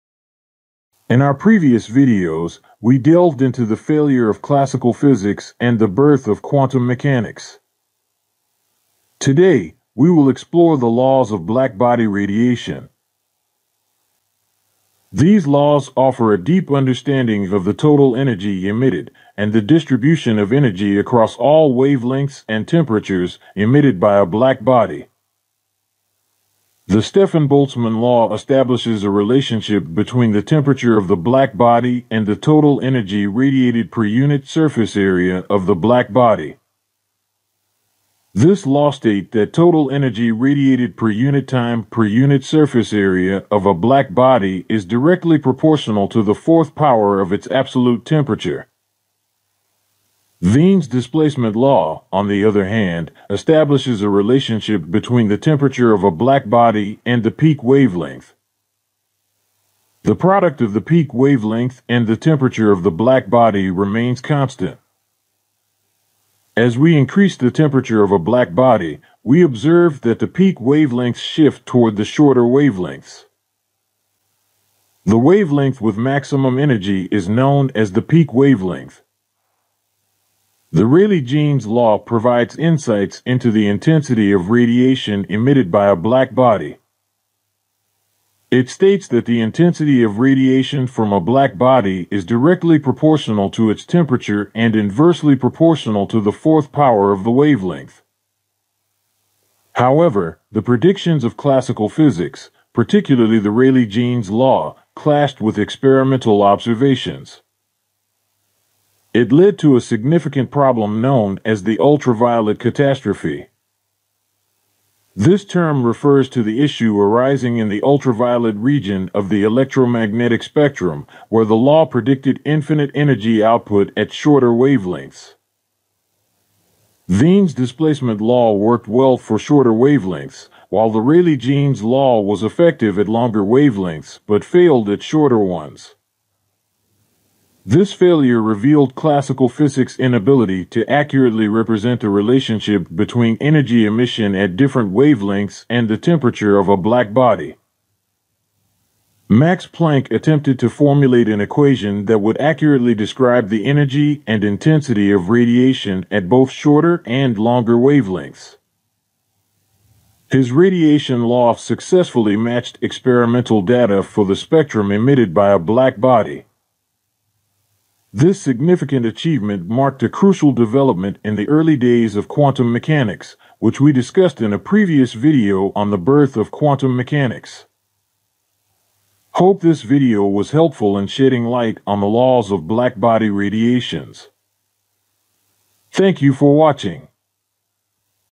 Wiens displacement law | What are the laws of radiation | Stefan boltzmann law
In this enlightening video, we delve into the fascinating world of black body radiation and the laws of black body radiation that govern it. Join us as we explore the key principles that challenged classical physics and paved the way for the birth of quantum mechanics. Discover the intriguing concepts of the Stefan-Boltzmann Law, Wien's Displacement Law, Rayleigh-Jeans Law, and Planck's Radiation Law, which provide profound insights into the behavior of black bodies at different temperatures and wavelengths. Witness the clash between classical physics and experimental observations, leading to the mysterious "ultraviolet catastrophe" and Max Planck's groundbreaking solution. Get ready for a journey into the depths of quantum mechanics and the profound implications of these laws on our understanding of the universe.

If you have any of the following questions in mind then this video is helpful for you.